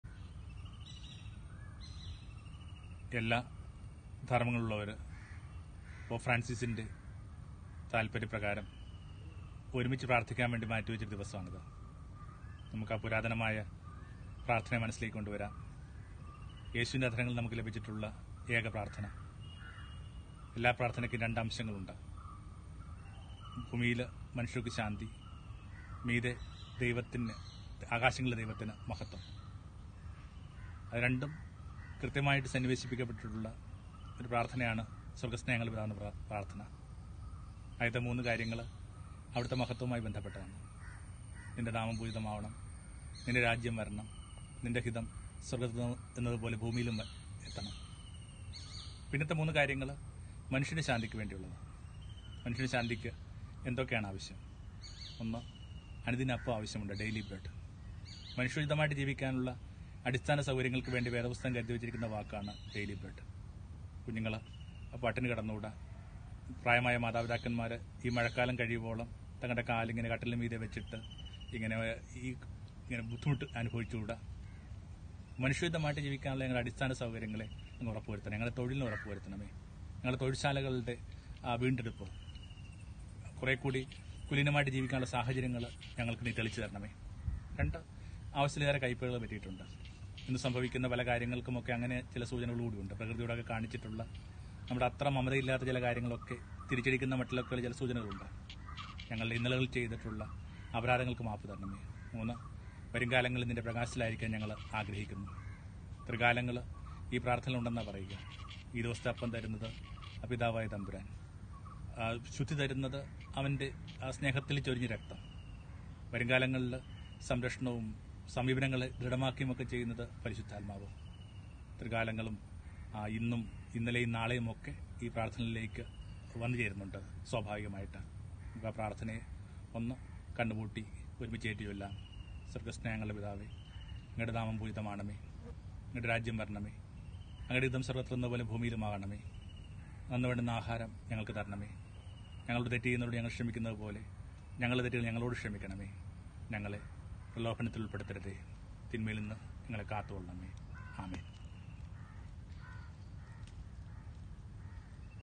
സ്വർഗസ്ഥനായ പിതാവേ | Bobby Jose Kattikadu | Pularvettam
2020 മേയ് 14-നു നടന്ന interreligious പ്രാർത്ഥനയോടനുബന്ധിച്ച് ബോബി ജോസ് കട്ടികാട് 'സ്വർഗസ്ഥനായ പിതാവേ' എന്ന പ്രാർത്ഥനയേക്കുറിച്ച്.